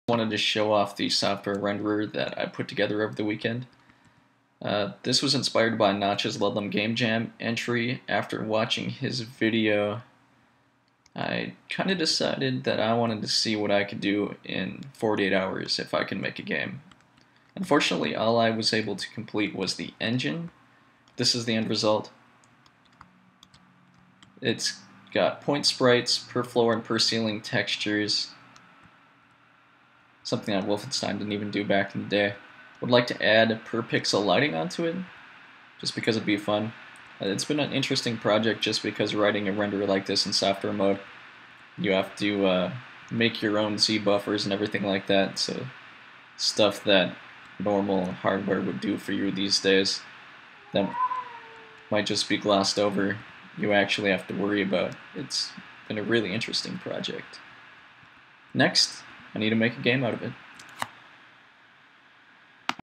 48 hour game engine
Quick demo of the raycast software renderer that I put together over the weekend. Watching Notch's live stream of him making his latest Ludlum 48 hour game jam entry inspired me to see if I could make something similar.